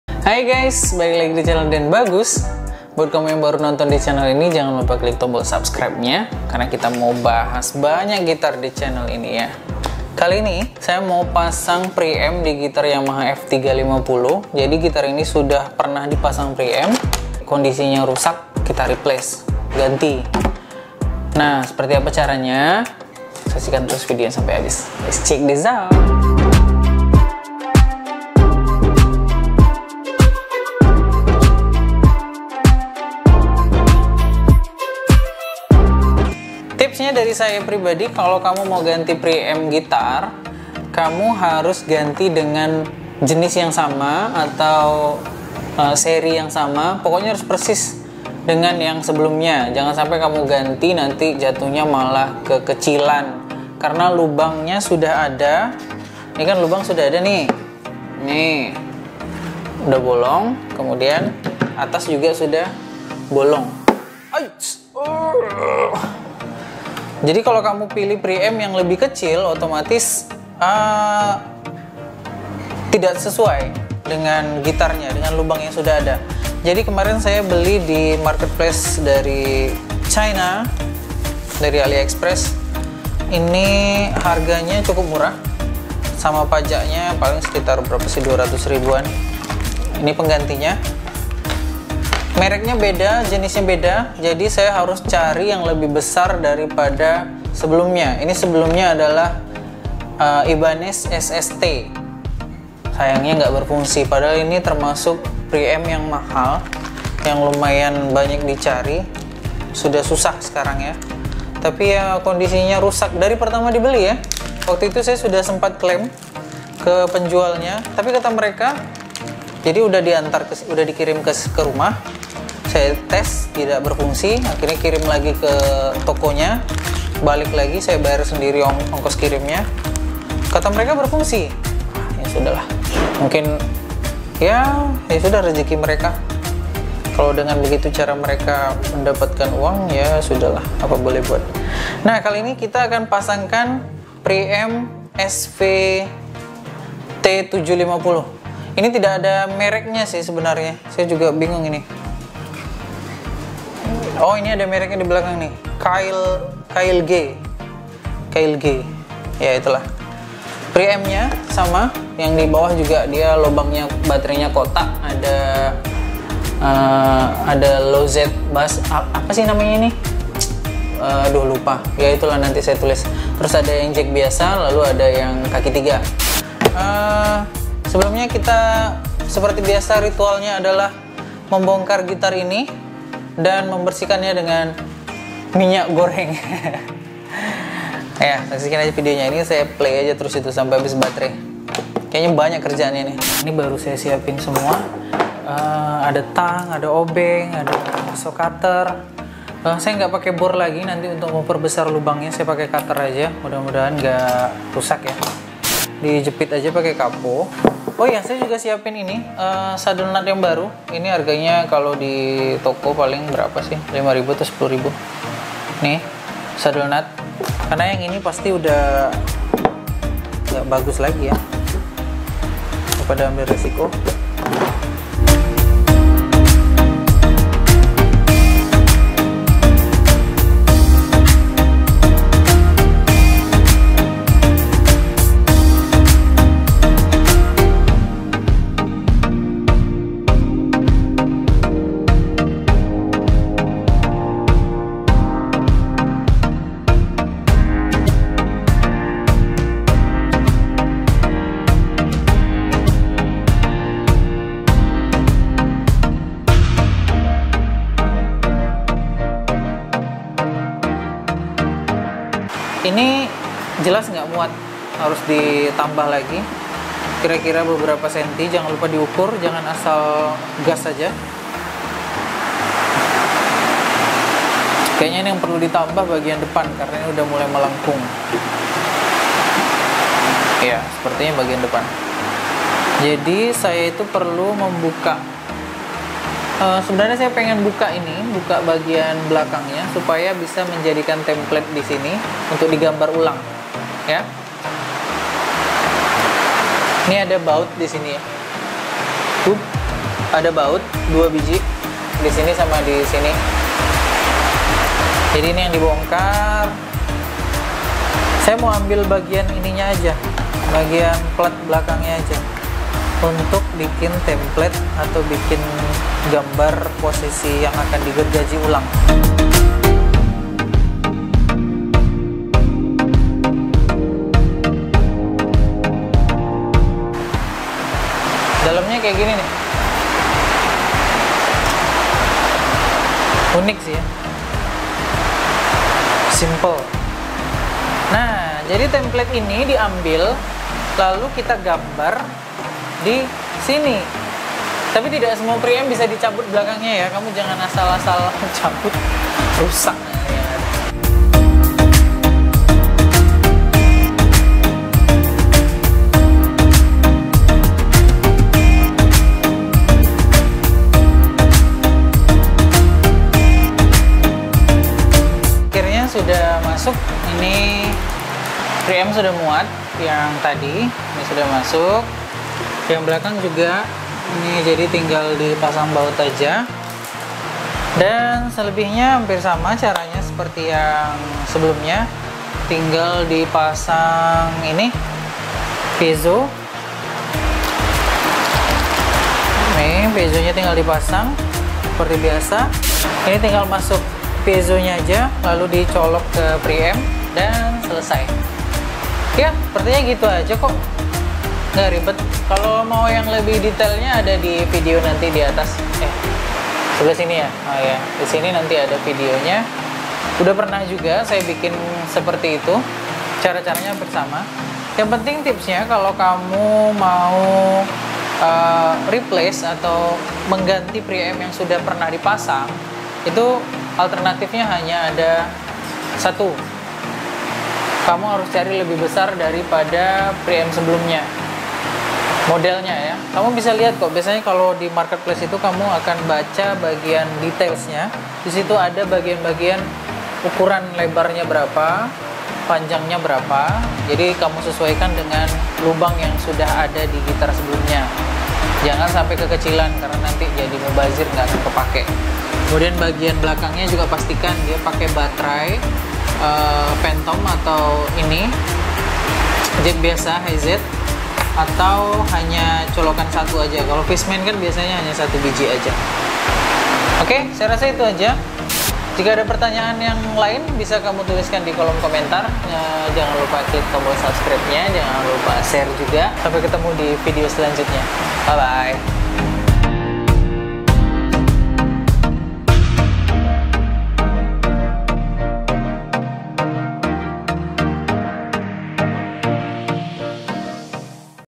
Preamp Gitar MANTAB TUTORIAL 006
Sebelumnya sudah pernah saya pasang preamp dari Ibanes SST yang ternyata rusak dr saat pembelian dan sekarang saya punya kesempatan untuk mengganti dengan preamp lain yaitu SV-T750 5 Band Equalizer

Beli disini (versi brand cowboy)

Terimakasih